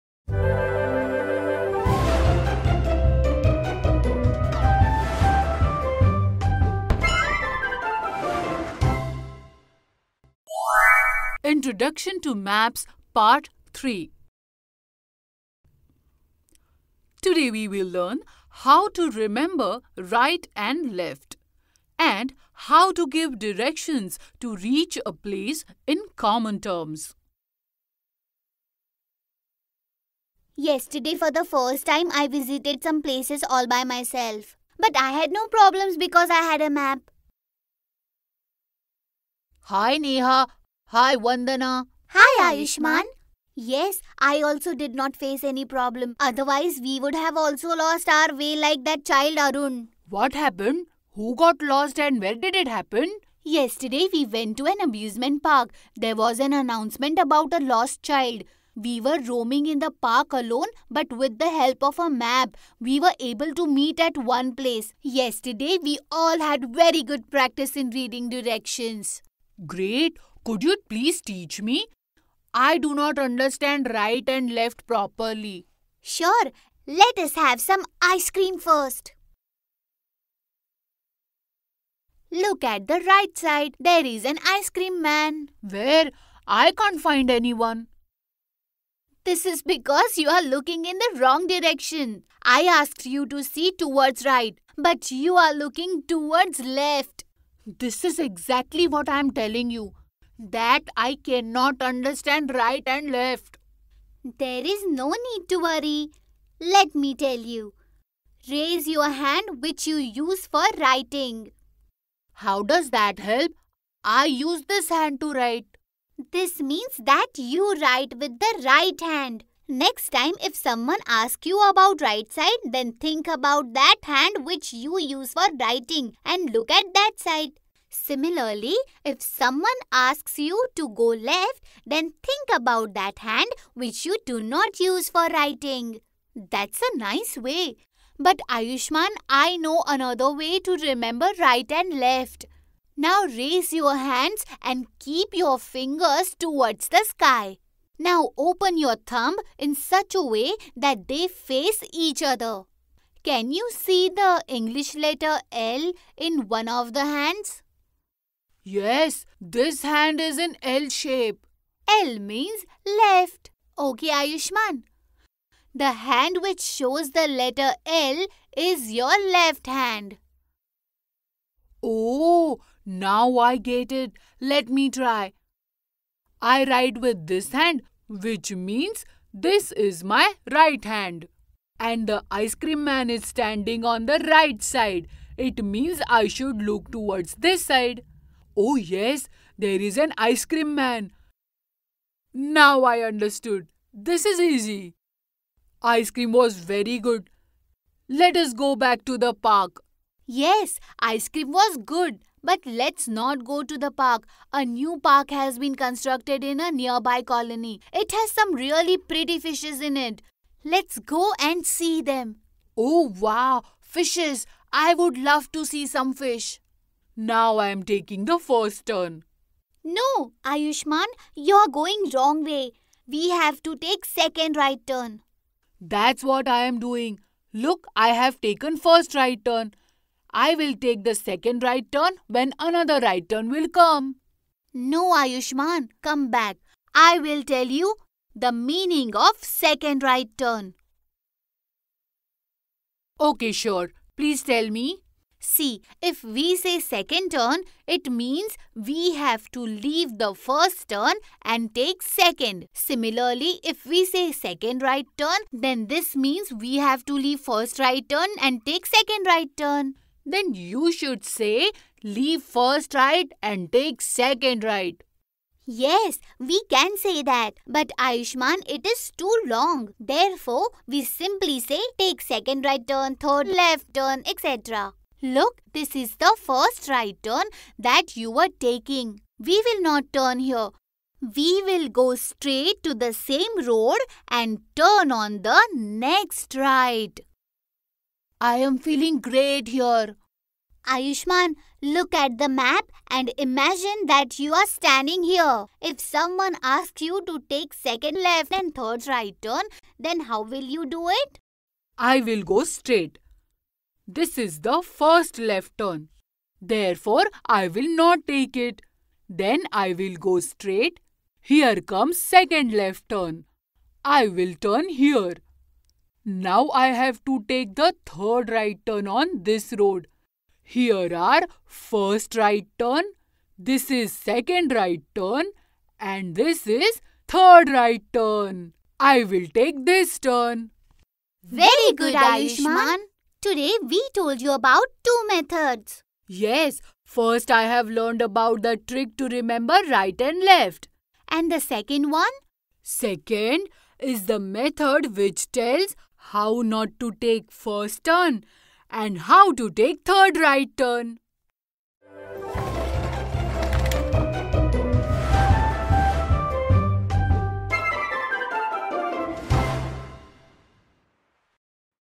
Introduction to Maps | Part 3/3 | English | Class 4
Introduction to Maps | Part 3/3 | English | Class 4 | NCERT

Maps | Shape and Space | Grade 4 | NCERT | The way the world looks

Welcome to part 3 of Maps. In this video, we will discuss various misconceptions and/or common errors related to Maps.